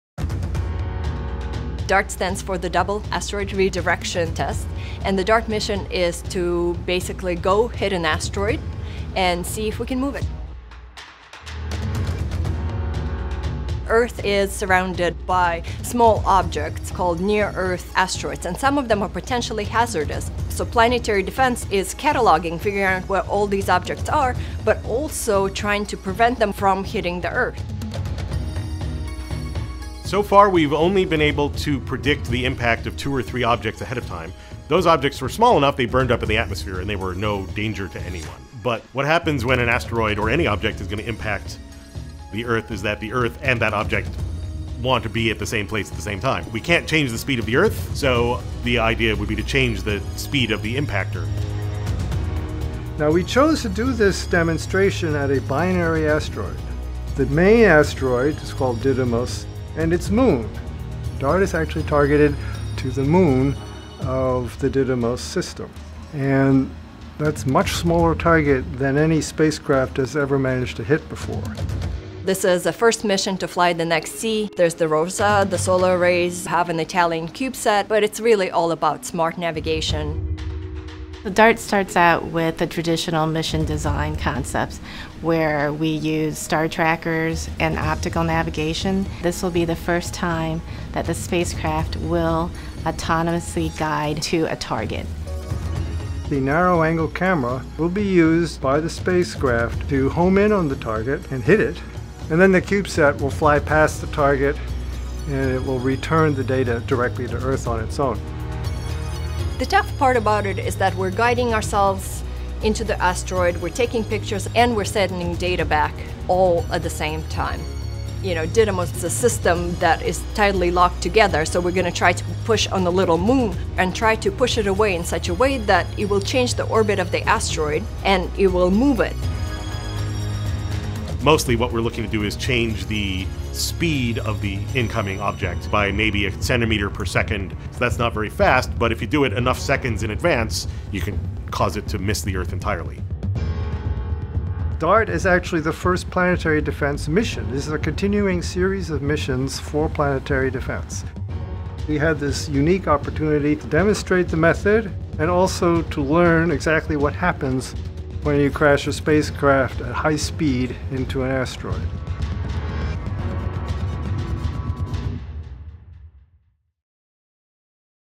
Defending the Planet: NASA’s DART Mission
Launching fall 2021, NASA's Double Asteroid Redirection Test (DART) will be the world’s first mission to test planetary defense techniques, demonstrating one mitigation method of asteroid deflection, called kinetic impact. DART will impact the small asteroid moonlet Dimorphos, which orbits a larger companion, Didymos, in a binary asteroid system to change its orbital period. Although neither asteroid poses a threat to Earth, the collision with Dimorphos enables researchers to demonstrate the deflection technique along with several new technologies, and collect important data to enhance our modeling and predictive capabilities for asteroid deflection. Those enhancements will help us better prepare should an asteroid ever be discovered as a threat to Earth.

The Johns Hopkins Applied Physics Laboratory manages the DART mission for NASA's Planetary Defense Coordination Office as a project of the agency’s Planetary Missions Program Office.